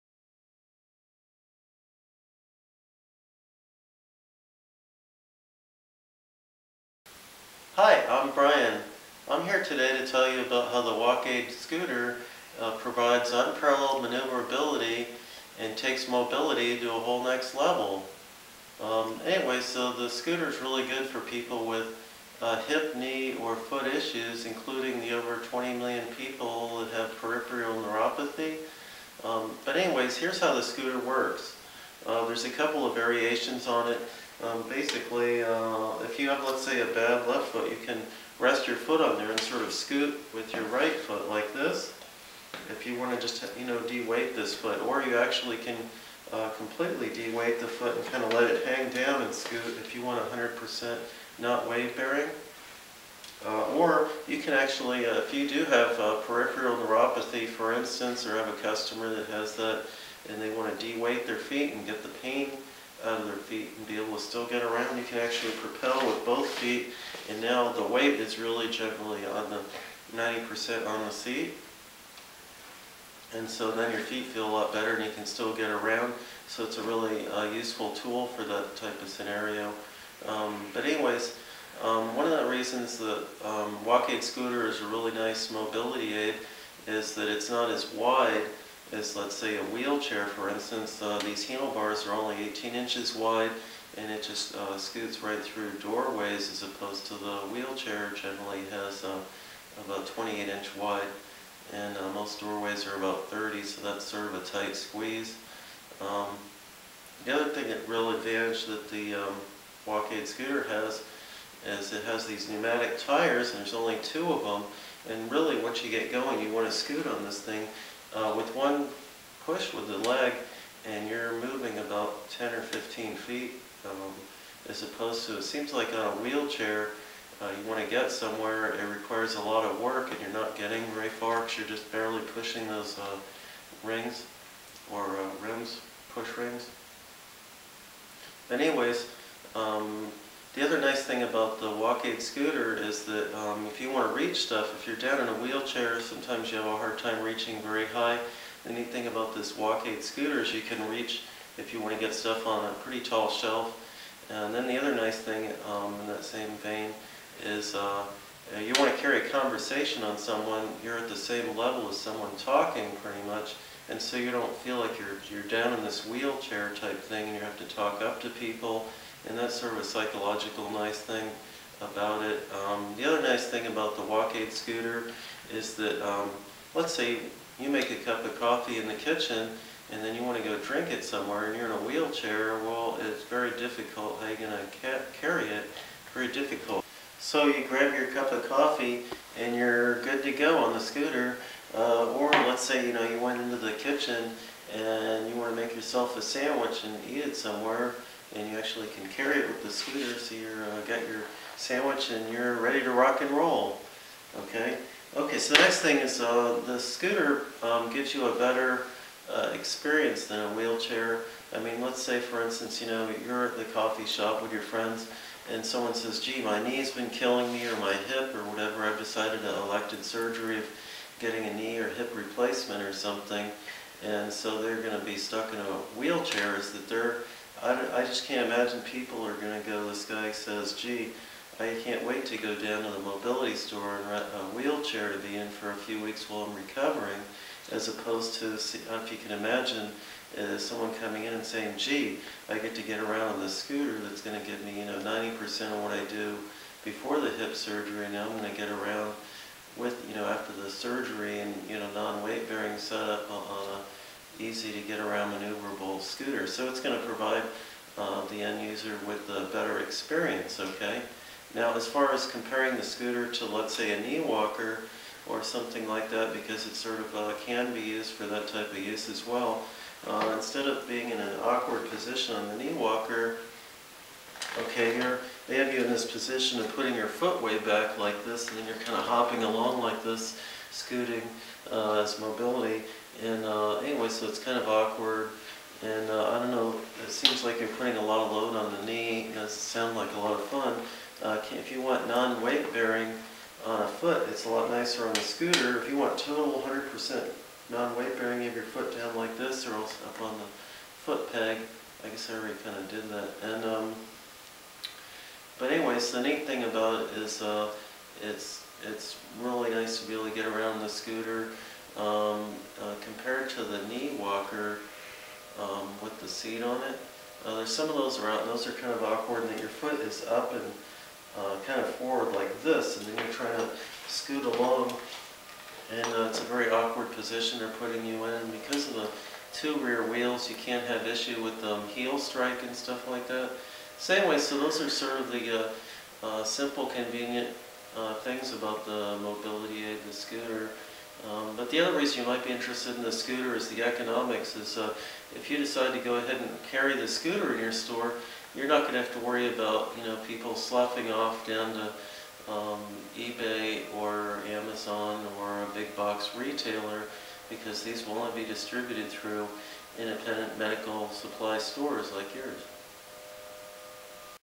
Knee Walker Knee Scooter Alternative | Walking Aid Medical Scooter Advantages
The Walk Aid Scooter provides unparalleled mobility for people who have trouble walking or standing because of hip, knee and foot ailments. If you have normal balance and have trouble getting around, this scooter might be fore you.